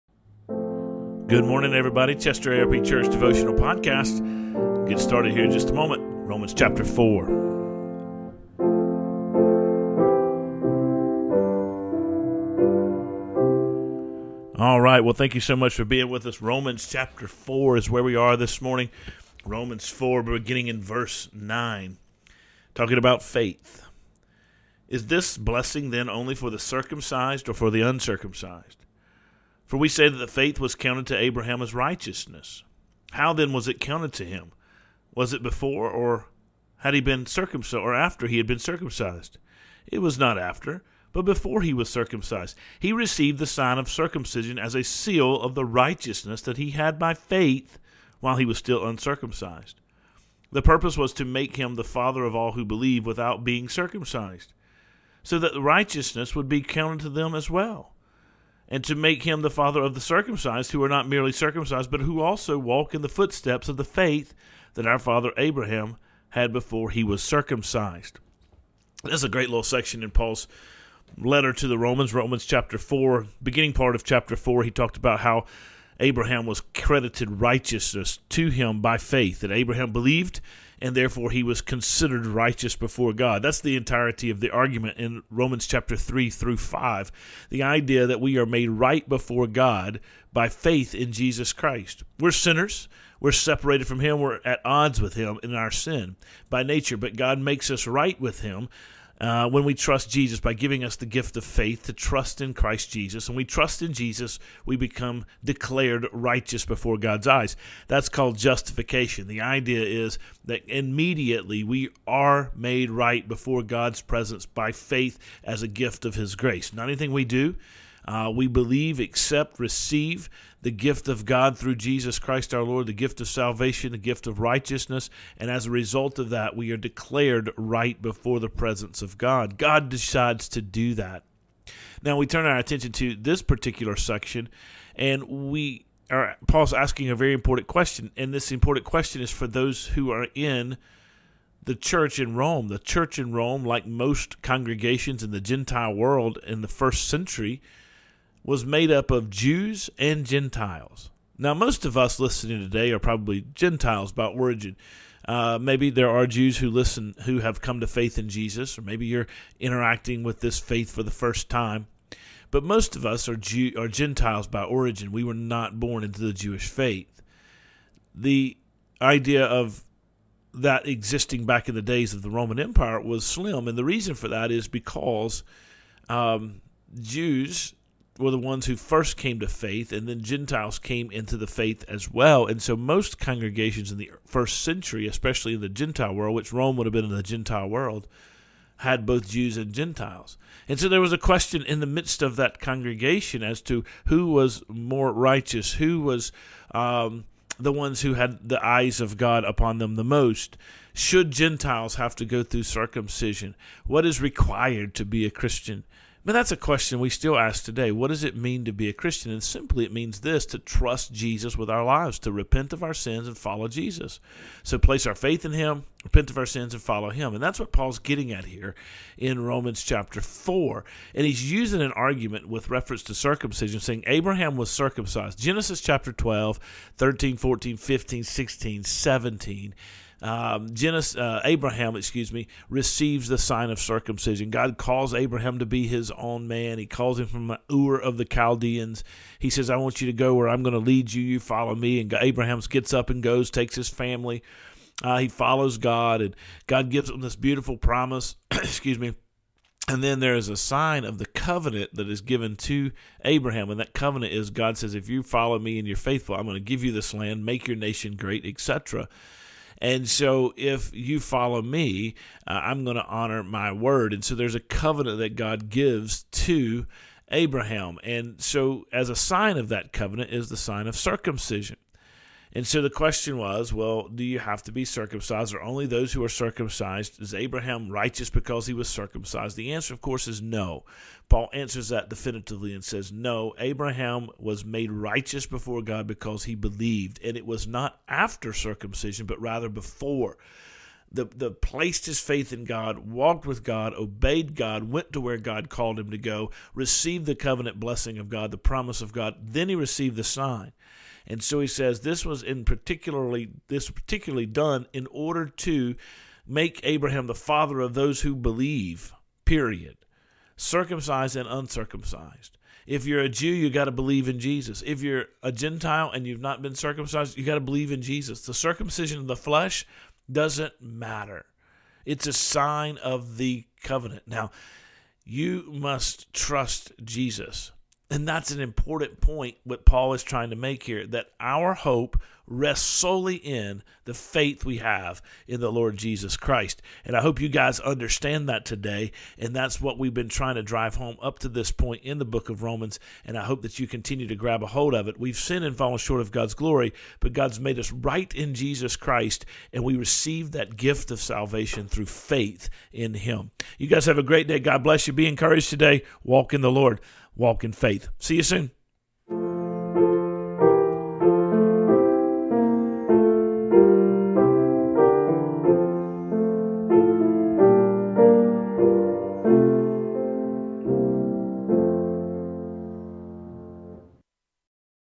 Abraham, Father of the Faithful -- Romans 4:9-12
Abraham is the father of those who've placed their faith in Jesus. We are made right before God by faith alone.